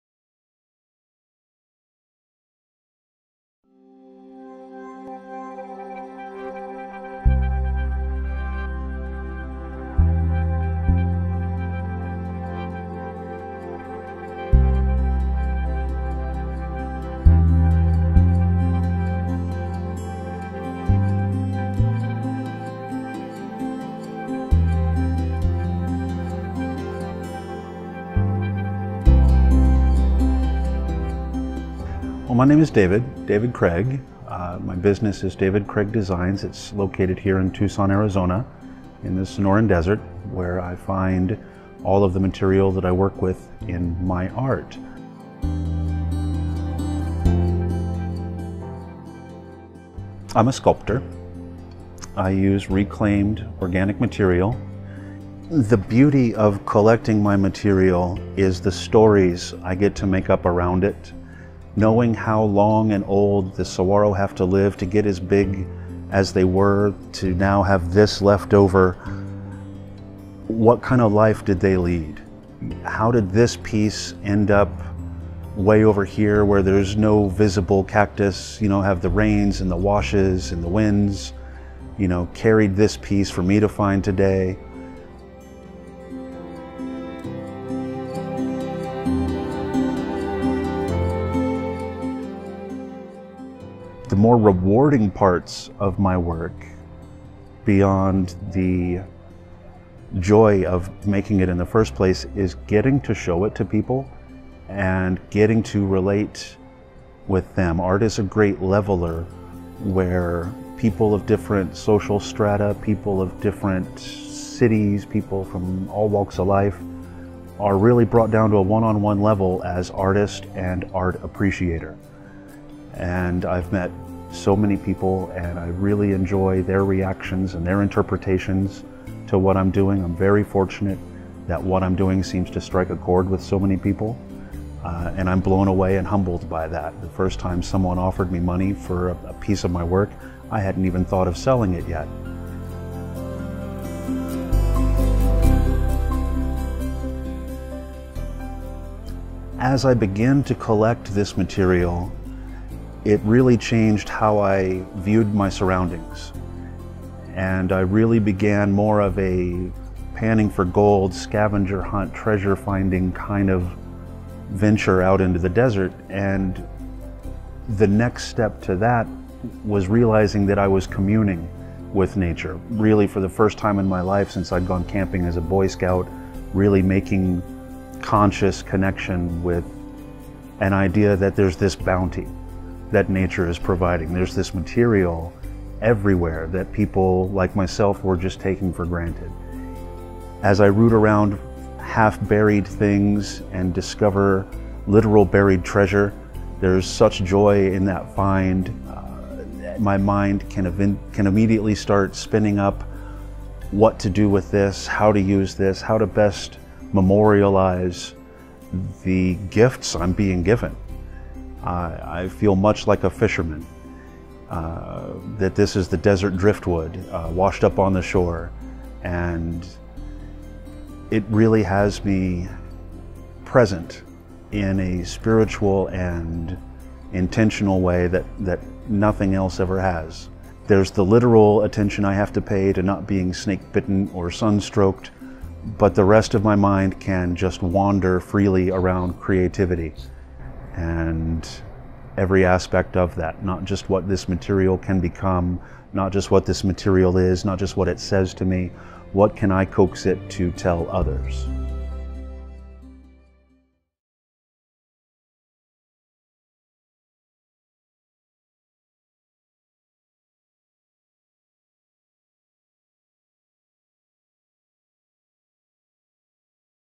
David Craig: Sculptor
“There’s this bounty that nature is providing, there’s this material everywhere that people like myself were just taking for granted.”

The natural world provides artists with many beautiful inspirations for their work. Some are more obvious than others and sometimes what nature gives requires a discerning eye. Tucson-based Sculptor David Craig uses natural materials as the base for his botanical-themed creations, specifically, he uses the husks of dead saguaro cacti.

This film is by Peter Fuhrman.
Produced by Peter Fuhrman.